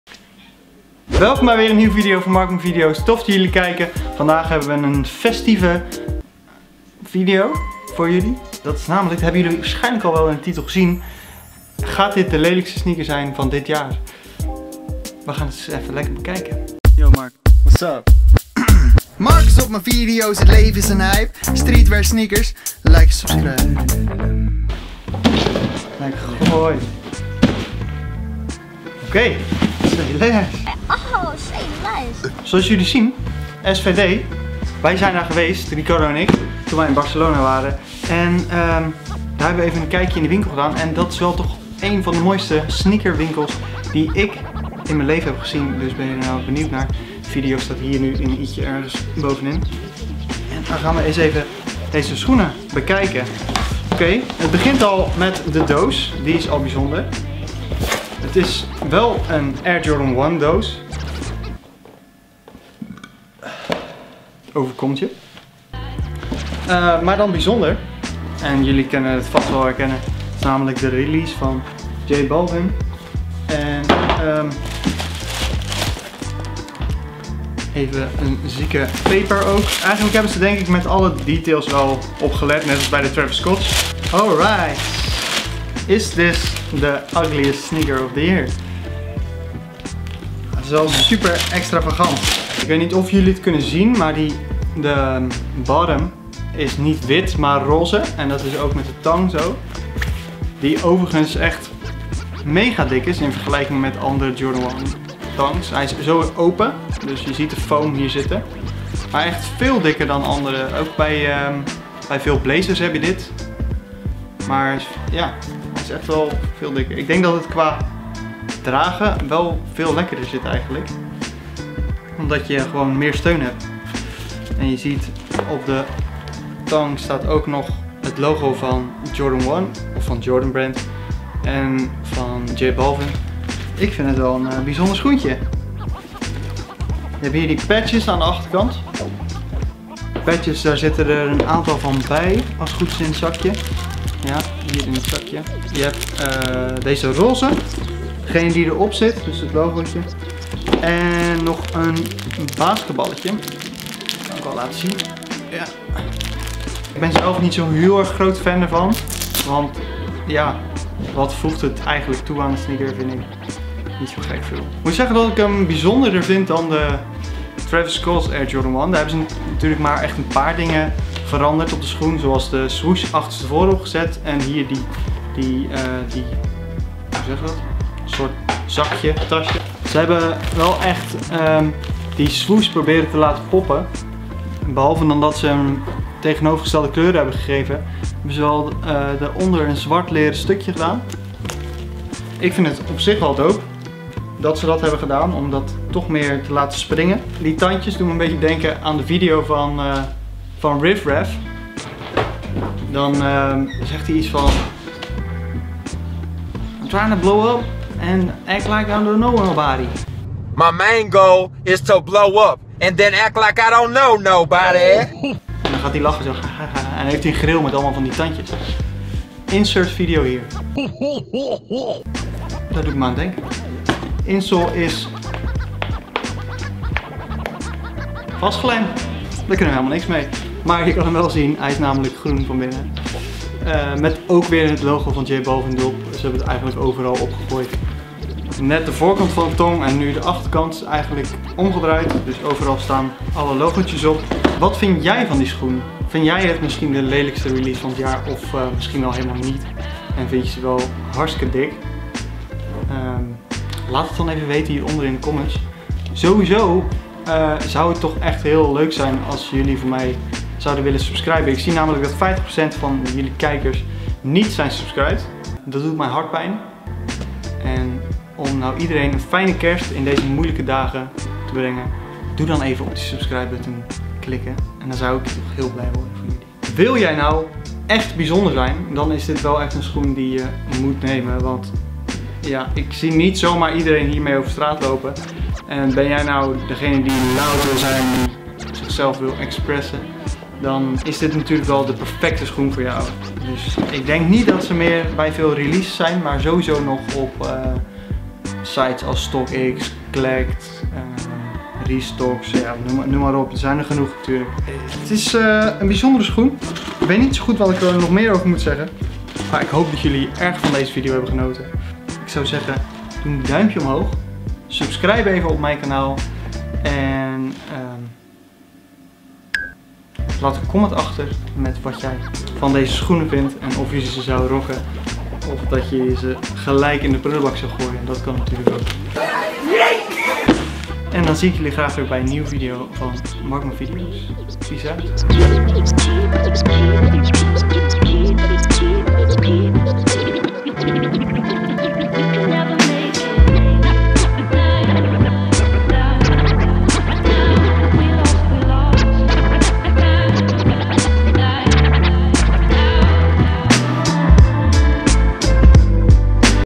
DE LELIJKSTE SCHOENEN VAN HET JAAR? || JORDAN 1 X J BALVIN REVIEW
Ik review de nieuwe Nike Air Jordan 1 J Balvin in deze video. Is dit de lelijkste sneaker van het jaar of is hij gewoon super vet?